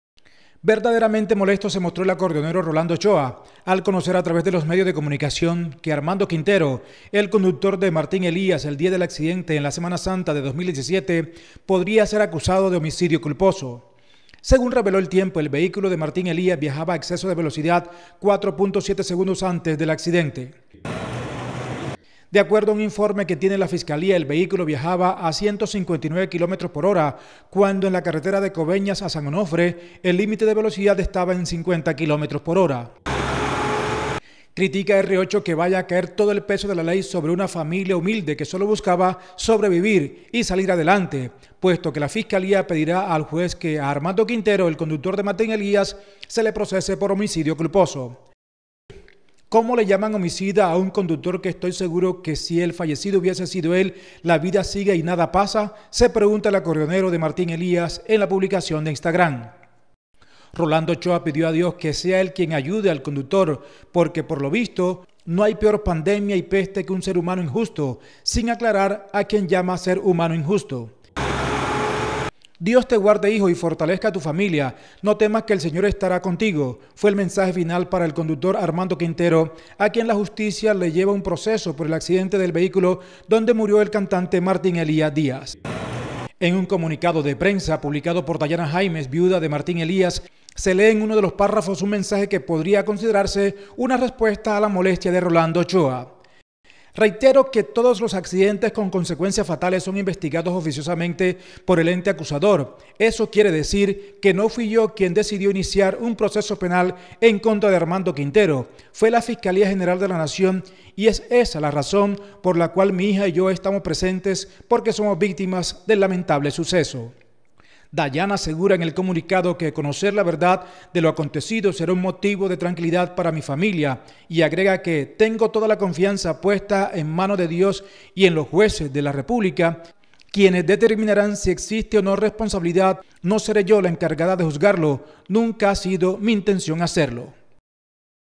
LA MOLESTIA DE R8 II MARTÍN ELÍAS II RTA NOTICIAS VALLEDUPAR
Verdaderamente molesto se mostró el acordeonero Rolando Ochoa, al conocer a través de los medios de comunicación que, Armando Quintero, el conductor de Martín Elías Díaz, el día del accidente en la semana santa de 2017, podría ser acusado de homicidio culposo.

Según reveló El Tiempo, el vehículo de Martín Elías viajaba a exceso de velocidad 4.7 segundos antes del accidente.

Según un informe que tiene la Fiscalía el vehículo viajaba a 159 kilómetros por hora, cuando en la carretera de Coveñas a San Onofre, el límite de velocidad estaba en 50 kilómetros por hora.

Critica R8 que vaya a caer todo “el peso de la ley sobre una familia humilde que sólo buscaba sobrevivir, y salir adelante”, puesto que la Fiscalía pedirá al juez que, a Armando Quintero, el conductor de Martín Elías se le procese por homicidio culposo.

“Cómo le llaman homicida a un conductor que estoy seguro que si el fallecido hubiera sido él, la vida sigue y no pasa nada”, se pregunta el acordeonero de Martín Elías en la publicación de Instagram.

Noticias de Valledupar, Cesar, Colombia
Música Vallenata
Cesar
Sur de La Guajira
Magdalena
Barranquilla
Bogotá